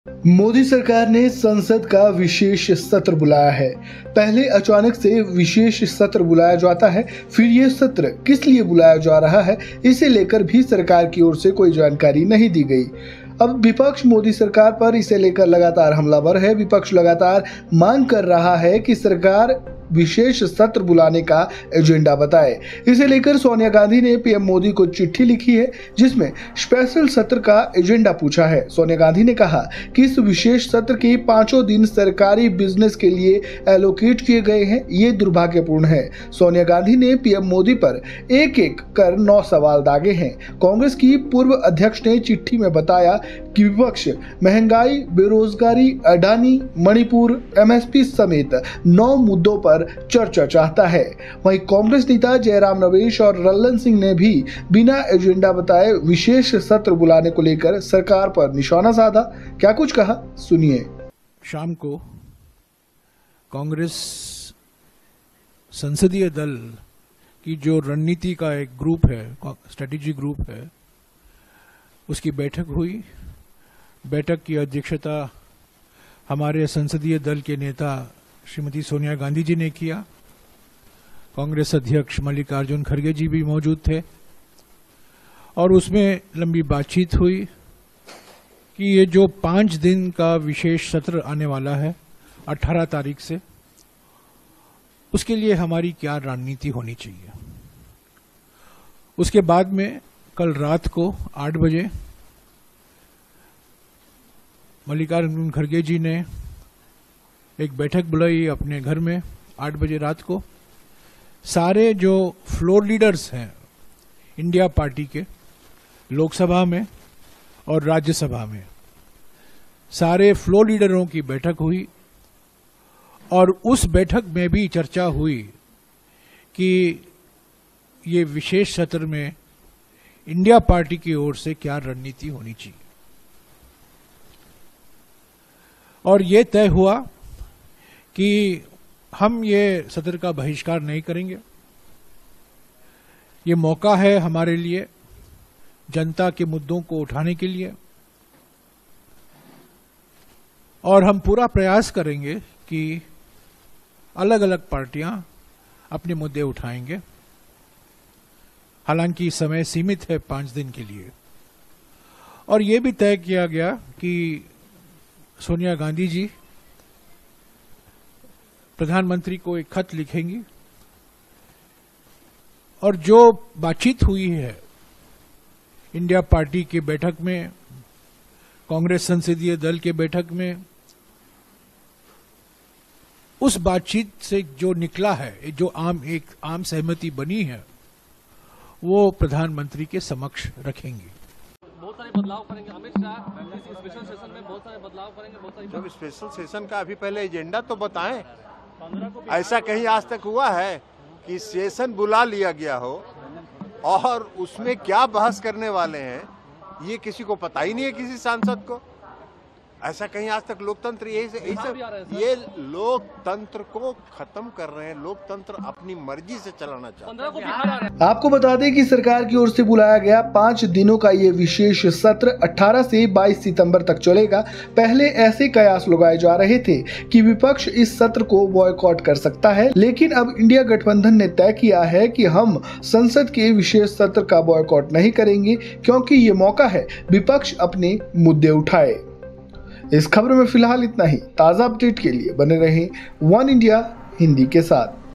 Sonia Gandhi ने Special Session पर PM Modi को लिखी चिट्ठी, क्या INDIA करेगा Boycott | वनइंडिया हिंदी
Parliament Special Session: मोदी सरकार (Modi Government) ने संसद का विशेष सत्र (Special Session) बुलाया है. पहले अचानक से विशेष सत्र बुलाया गया... फिर ये सत्र किस लिए बुलाया जा रहा है इसे लेकर भी सरकार की ओर से कोई जानकारी नहीं दी गई,... जिसे लेकर विपक्ष मोदी सरकार पर हमलावर है. विपक्ष लगातार मांग कर रहा है कि सरकार इसका एजेंडा बताए. इसे लेकर सोनिया गांधी (Sonia Gandhi) ने पीएम मोदी (PM Modi) को चिट्ठी भी लिखी है, जिसमें स्पेशल सत्र का एजेंडा पूछा है और क्या कुछ कहा चलिए जानते हैं.